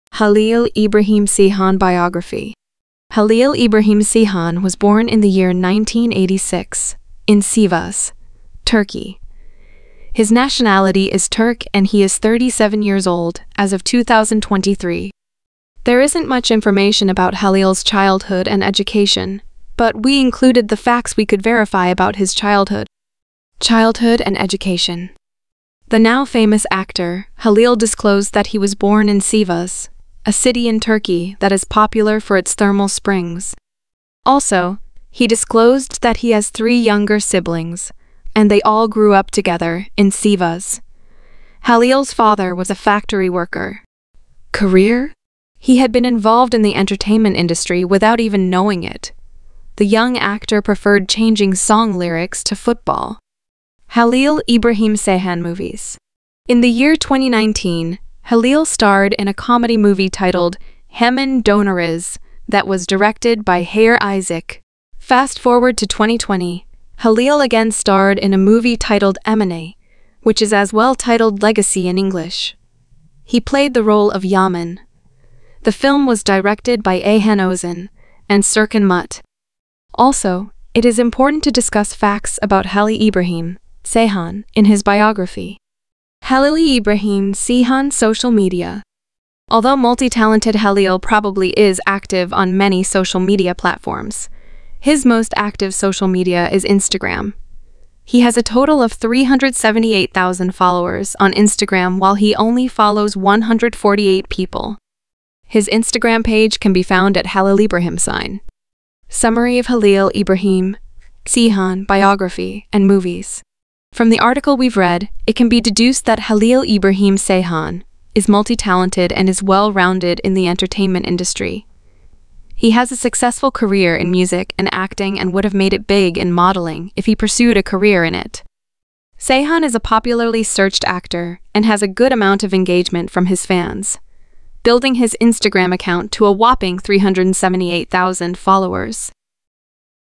Halil Ibrahim Ceyhan Biography all details | facts you don't know about halil
Halil Ibrahim Ceyhan was born in the year 1986, in Sivas, Turkey. His Nationality is Turk and he is 37 years old as of 2023.
There isn’t much information about Halil’s Childhood and Education but we included the facts we could verify about his childhood.
Childhood and Education
The Now famous actor, Halil disclosed that he was born in Sivas, a city in Turkey that is popular for its thermal springs.
Also, he disclosed that he has 3 younger siblings and they all grew up together in Sivas. Halil’s father was a factory worker.
Career
Unknowingly to Halil, his career started at the early stage of his life, while he was a child. Recall that he would change song lyrics and mimick Turkish actors when he was younger.
He had been involved in the entertainment industry without even knowing it. The young actor preferred changing song lyrics to football.
Halil Ibrahim Ceyhan Movies
In the year 2019, Halil starred in a Comedy movie titled “Hemen Doneriz” that was directed by Haydar Isik.
Fast forward to 2020, Halil again starred in a movie titled “Emanet” which is as well titled “Legacy” in English. He played the role of Yaman. The film was directed by Ayhan Ozen and Serkan Mut
Also, it is important to discuss facts about Halil Ibrahim Ceyhan in his biography
Although multi-talented Halil probably is active on many social media platforms, his most active social media is Instagram. He has a total of 378,000 followers on Instagram while he only follows 148 people. His Instagram page can be found at Halilibrahimceyhn.
Summary of Halil Ibrahim Ceyhan Biography and Movies
From the article we’ve read, it can be deduced that Halil Ibrahim Ceyhan is multi-talented and is well-rounded in the Entertainment Industry. He has a successful career in Music and Acting and would have made it big in Modeling if he pursued a career in it.
Ceyhan is a popularly searched actor and has a good amount of engagement from his fans, building his Instagram account to a whopping 378,000 followers.